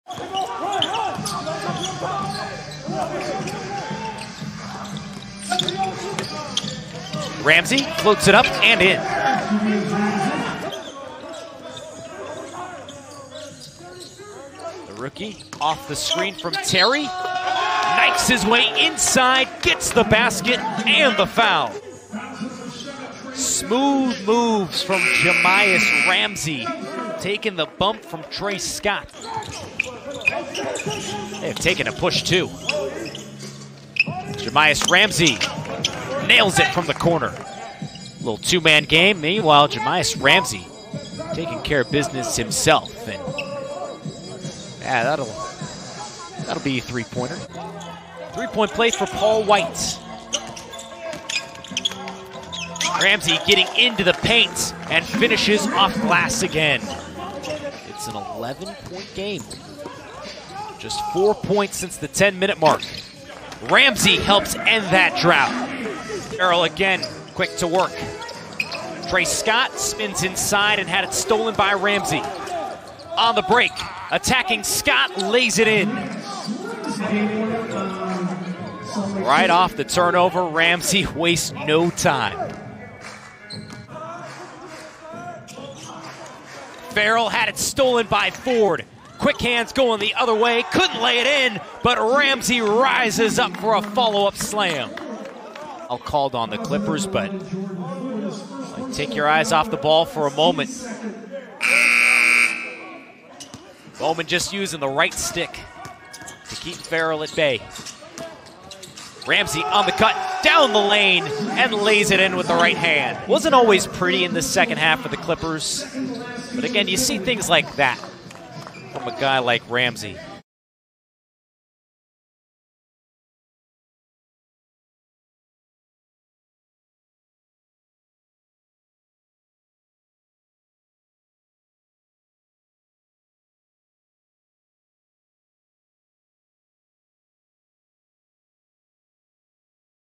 Jahmi'us Ramsey Scores 21 PTS In Agua Caliente Win
Jahmi'us Ramsey tied his season-high in scoring with 21 PTS as the Agua Caliente Clippers beat the Salt Lake City Stars, 100-87, on Tuesday.